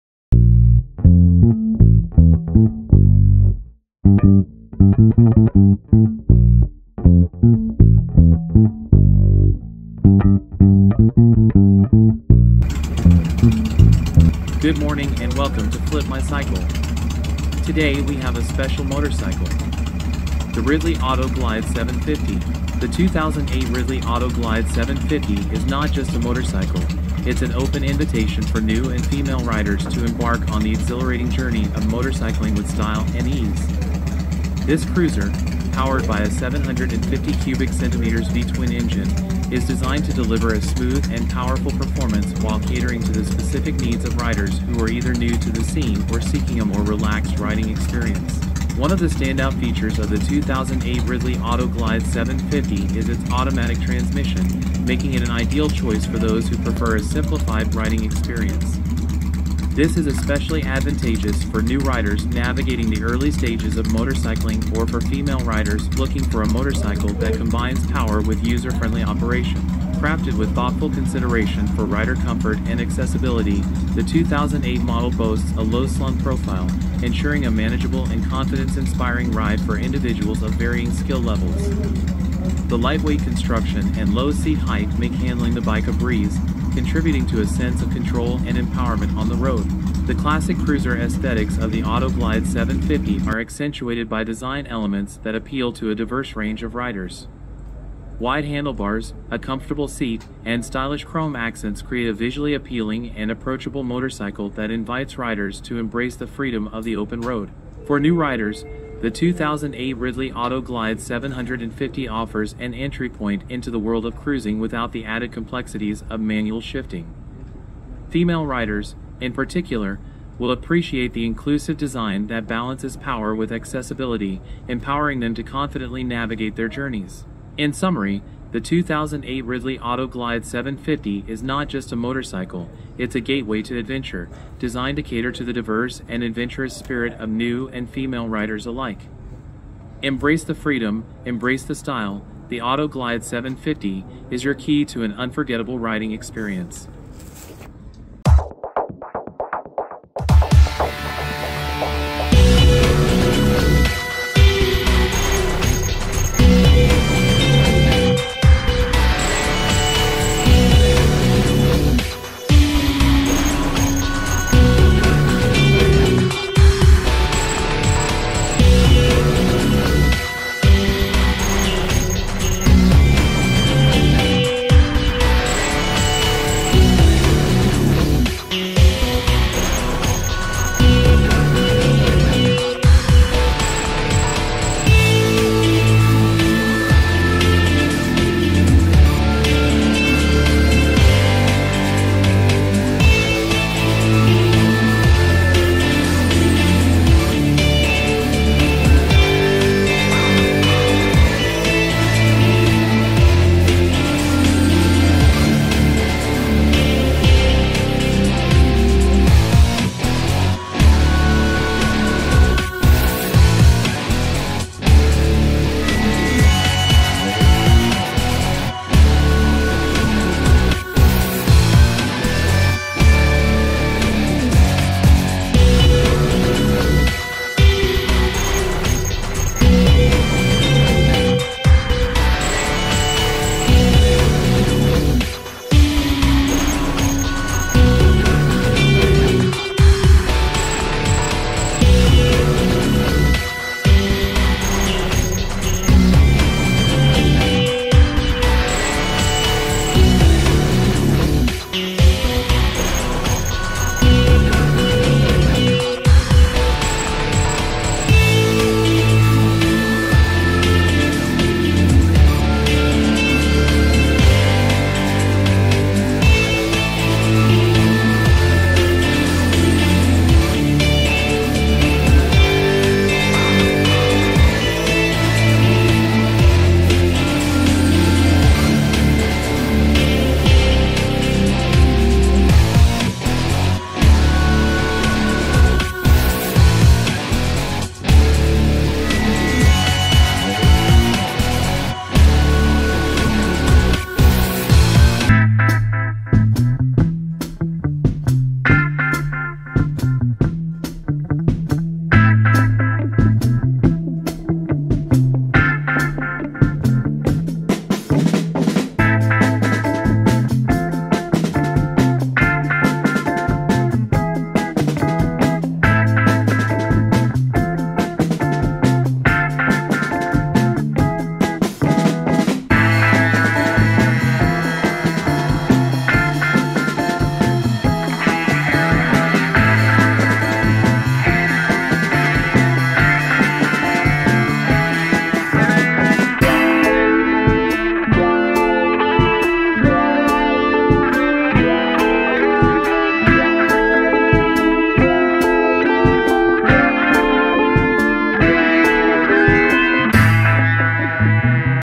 2008 Ridley Auto Glide
Riding the Ridley Auto Glide 750 is a captivating experience that seamlessly blends power, comfort, and accessibility. As you settle into the low-slung saddle, the ergonomic design of the bike becomes immediately apparent, offering a relaxed riding position that feels natural and confidence-inspiring. The 2008 Ridley model, with its lightweight construction and low seat height, ensures that riders of various skill levels, including beginners and female riders, can easily handle the motorcycle.

The heartbeat of the Auto Glide 750 is its 750cc V-twin engine, providing a potent surge of power with each twist of the throttle. The automatic transmission eliminates the need for manual shifting, allowing riders to focus on the sheer joy of the ride without the complexities associated with traditional cruisers.

Navigating city streets or open highways becomes a seamless dance between rider and machine. The wide handlebars offer precise control, and the bike's responsive nature makes maneuvering through traffic or cruising along scenic routes a delight. The absence of a clutch lever and gear shifter simplifies the riding process, making it an ideal choice for those new to motorcycling.

As you open up the throttle, the Auto Glide 750 effortlessly accelerates, delivering a smooth and exhilarating performance. The wind in your face, the rumble of the V-twin beneath you, and the classic cruiser aesthetics create an immersive experience that taps into the essence of freedom on two wheels.

The comfortable seat, adorned with stylish detailing, invites riders to embark on extended journeys with ease. Whether you're a seasoned rider or a newcomer, the Ridley Auto Glide 750 offers an accessible and enjoyable riding experience, transcending the boundaries of conventional cruisers. It's not just a motorcycle; it's a gateway to adventure, inviting you to embrace the open road with style and confidence.